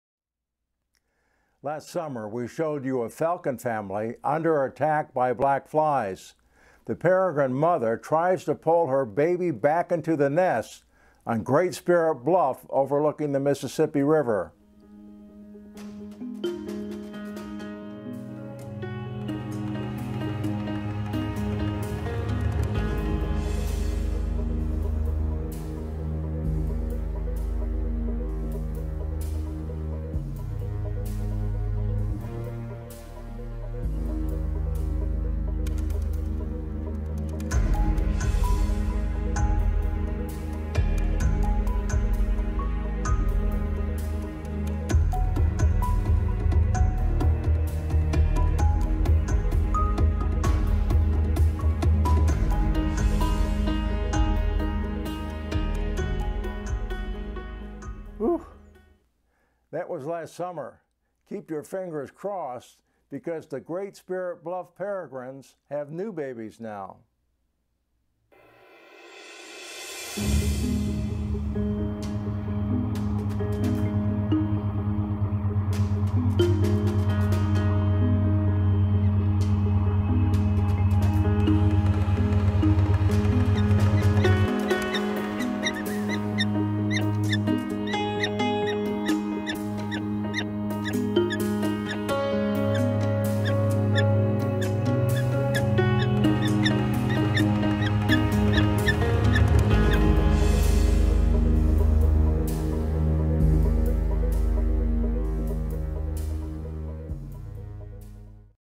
Black flies can kill young birds
In late spring and early summer, black flies emerge from rivers and make life miserable for man, beasts and birds. Females sucks out our blood and leave painful welts. Here in Wisconsin, I've seen them invade the nests of eagles, whooping cranes and peregrine falcons. To avoid the flies, nest babies sometimes jump out like the poor souls in the World Trade Center. Previous video